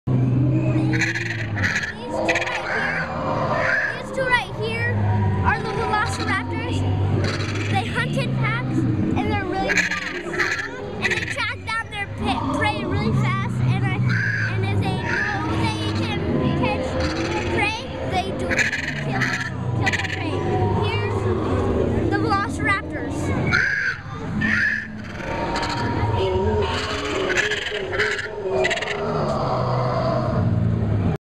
The Velociraptor Dinosaur
Name: Velociraptor
Means: Swift Robber
Found: Asia
Size: 6 feet long

Some think Velociraptors hunted in packs, What do you think?